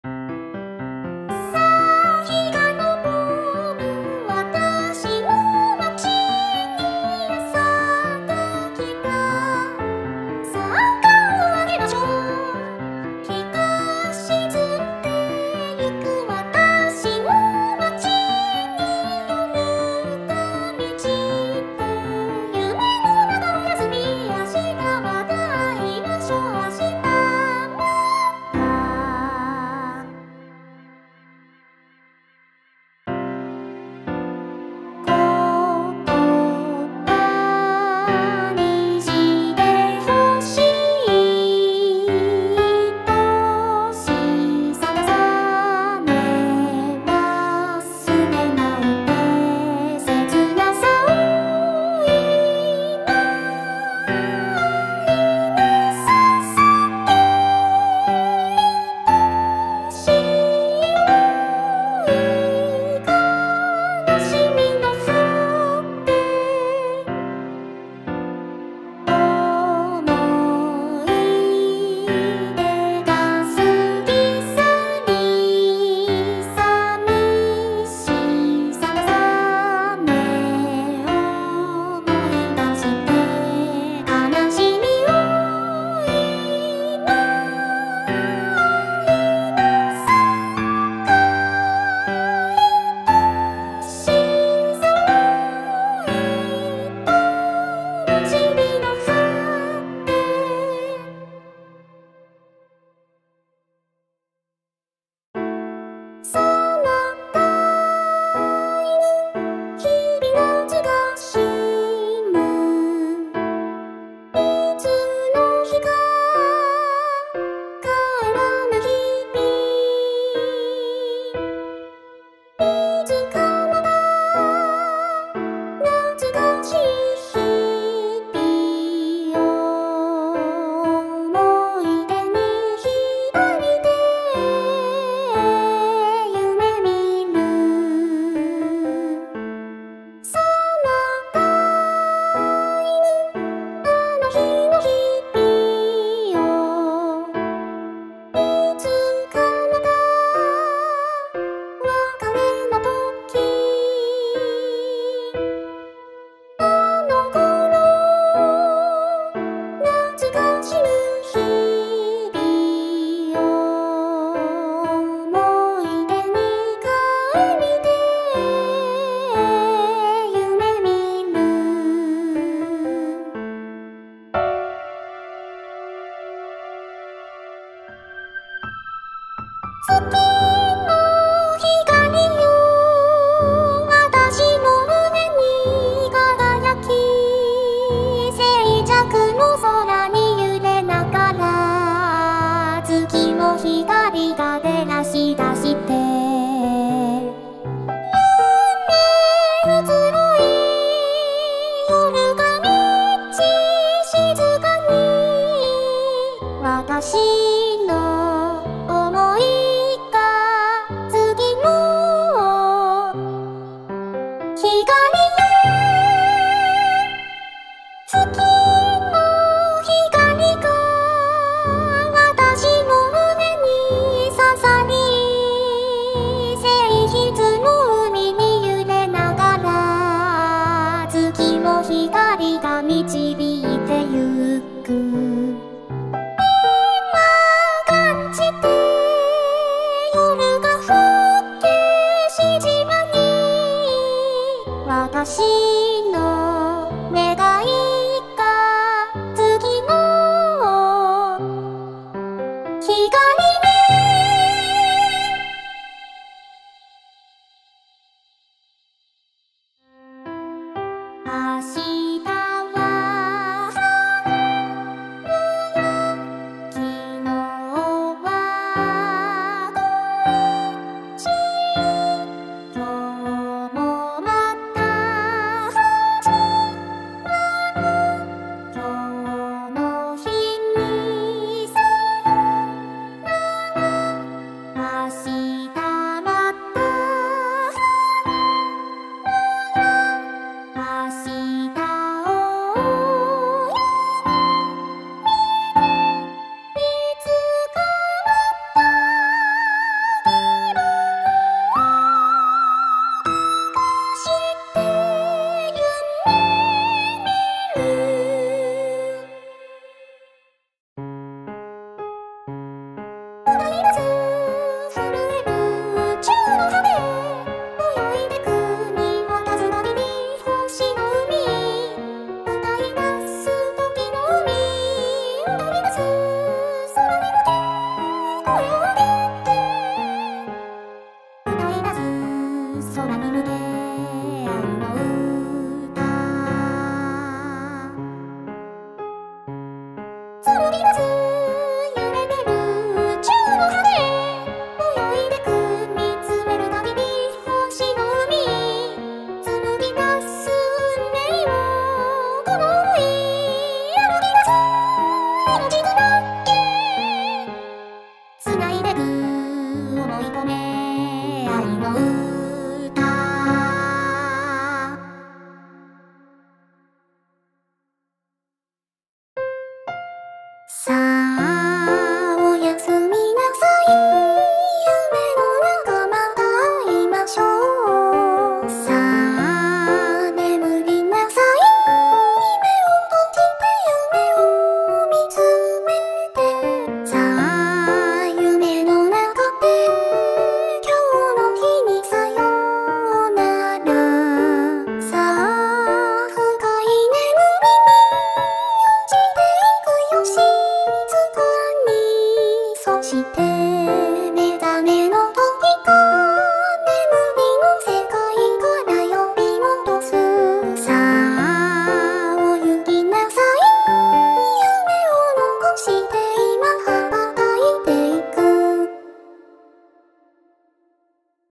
初音ミクが歌うクラシック曲メドレー
クラシック曲にオリジナルの詩をつけて、初音ミクに歌わせました。